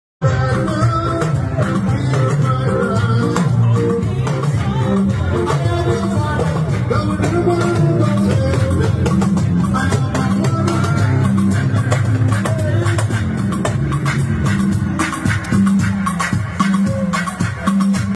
🎂🎈Former Governor of Lagos, Mr Akinwunmi Ambode clocks 60
Governor Sanwo-Olu arrives at the 60th Birthday Celebration of his predecessor, Akinwunmi Ambode today in Ikoyi, Lagos.